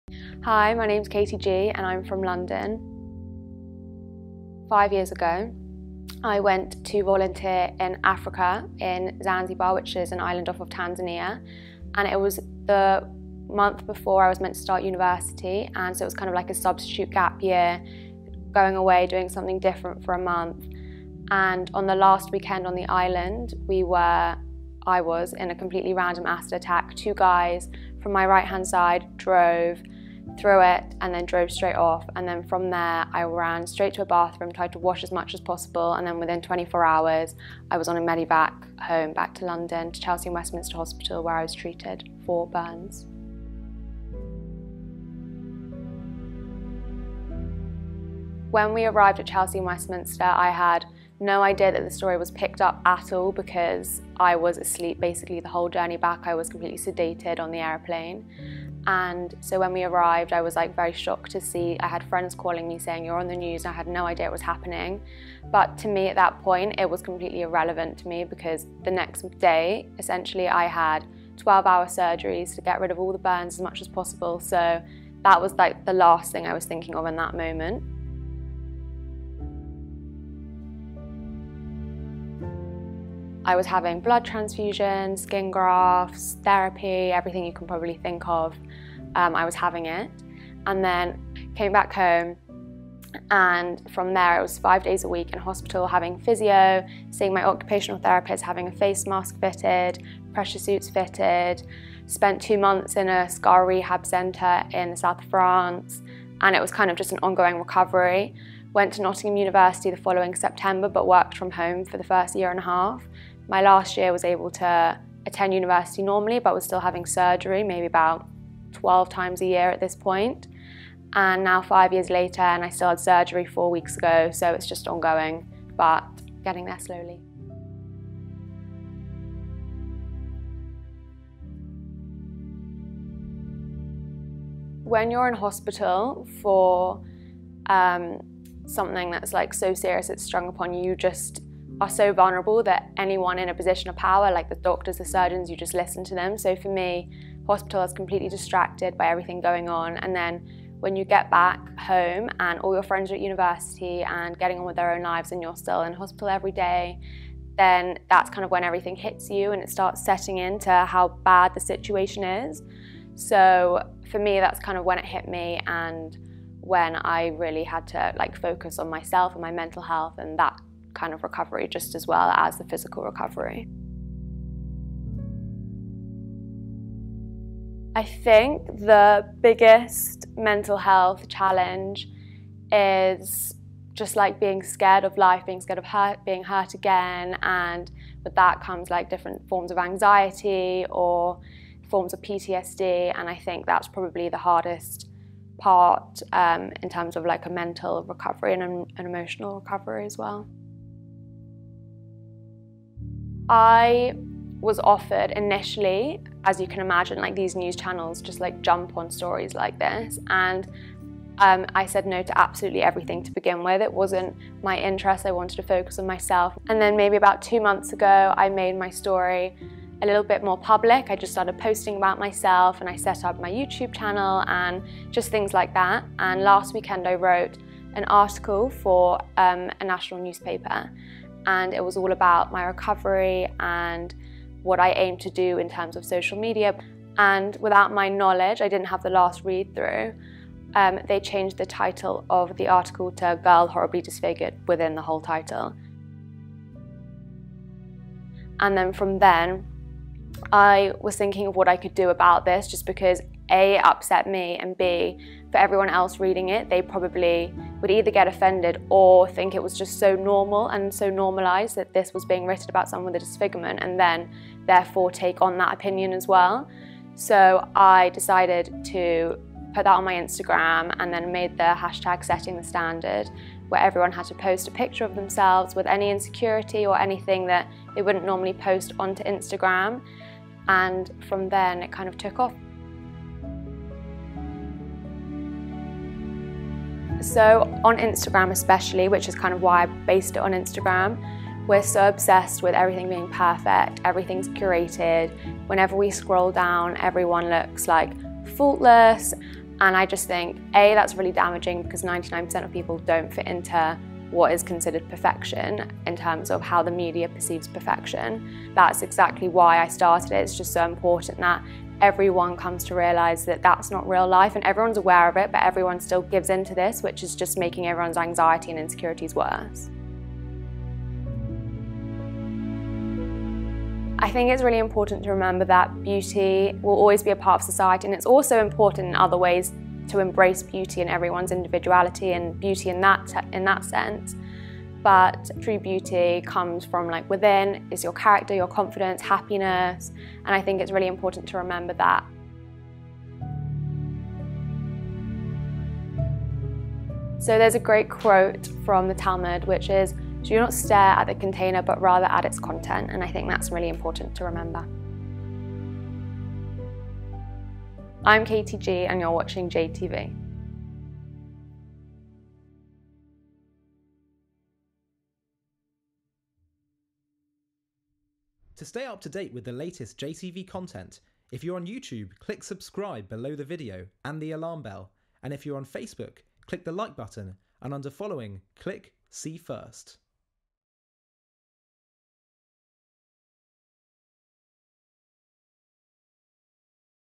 I survived an acid attack. Now I want to change our perception of beauty.
Meet Katie Gee. She wants to change beauty standards and expectations in society. "Do not stare at the container, but rather at its content" - The Talmud.